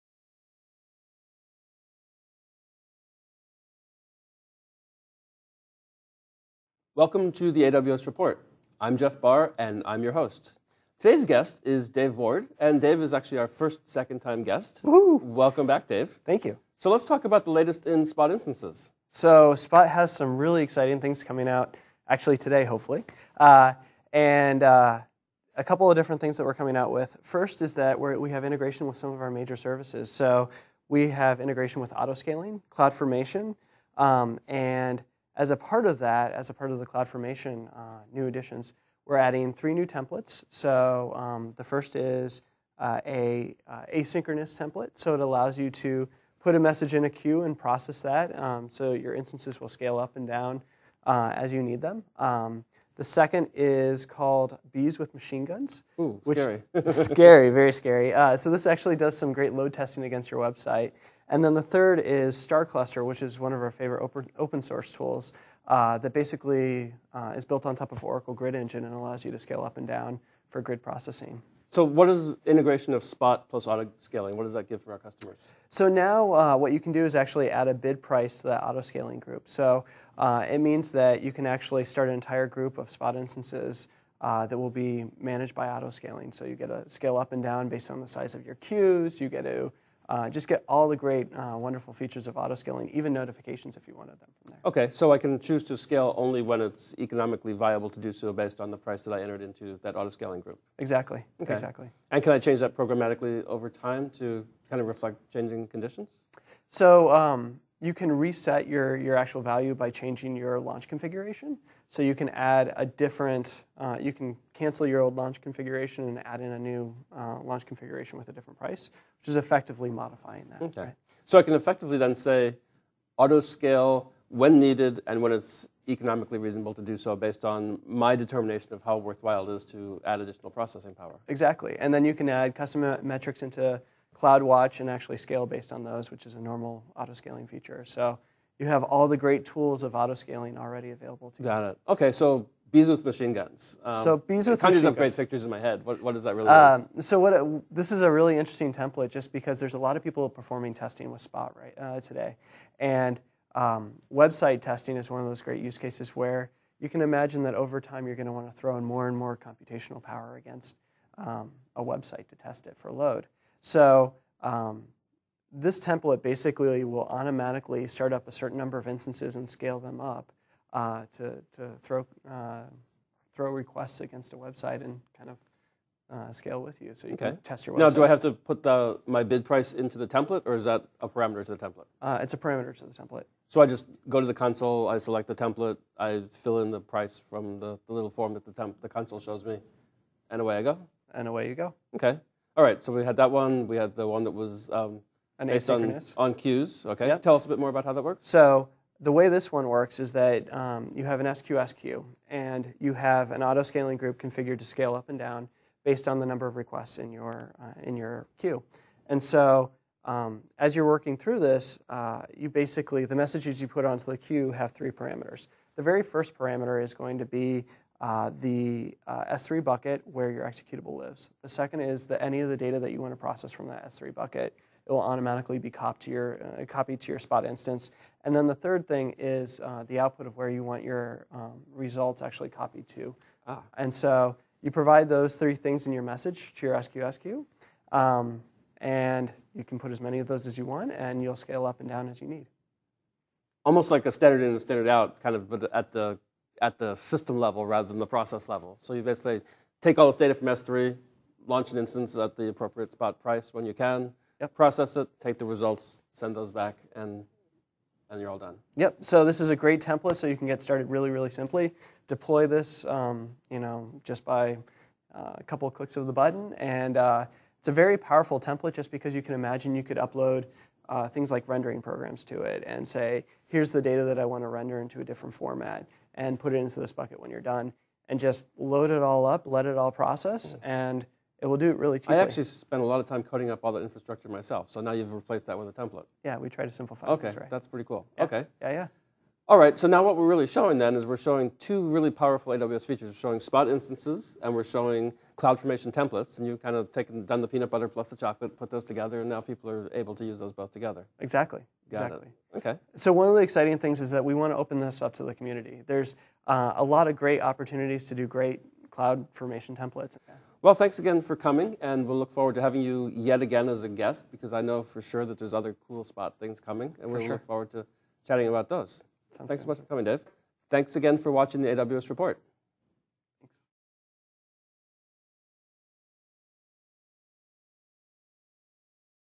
AWS Report with Dave Ward
AWS Report with Dave Ward discuss Spot Instances Integration with Auto Scaling and CloudFormation.